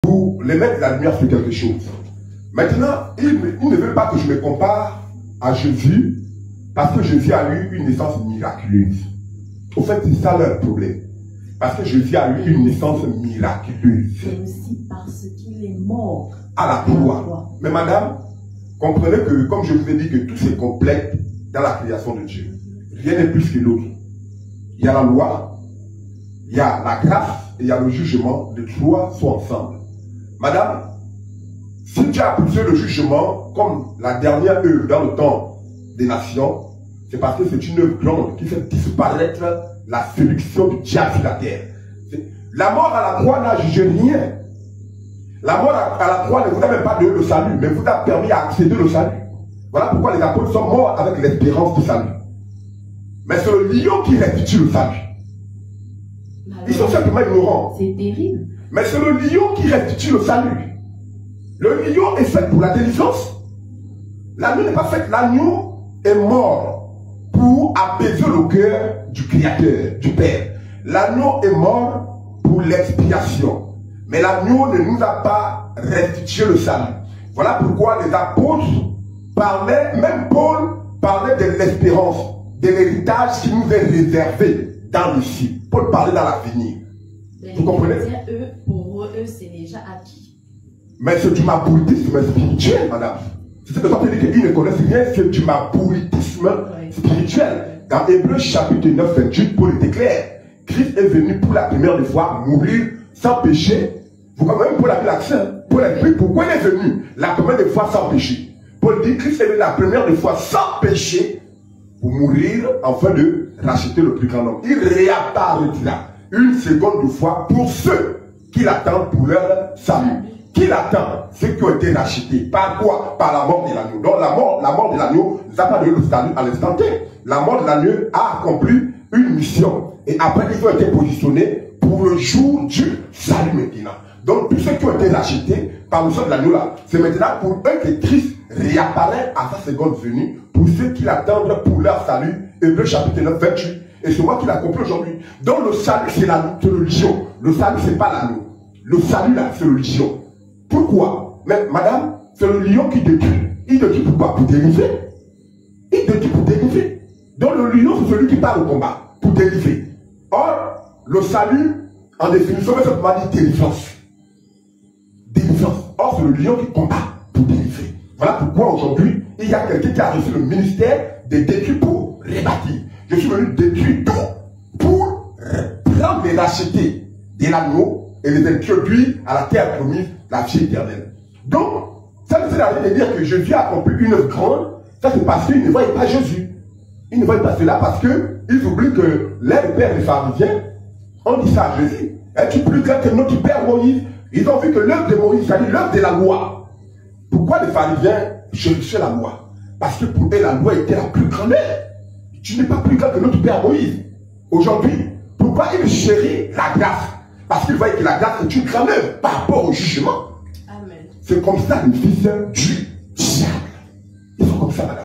C'EST LE LION QUI RESTITUE LE SALUT
Voici le lien de la vidéo complète: "QUESTION D'UN INTERNAUTE : SI TON CHRIST N'EST PAS APPELE A MOURIR A LA CROIX, NE NOUS FATIGUEZ PAS" - Seigneur Jonas Souanguele  👇👇

🔴 Prêché le 07 AVRIL 2023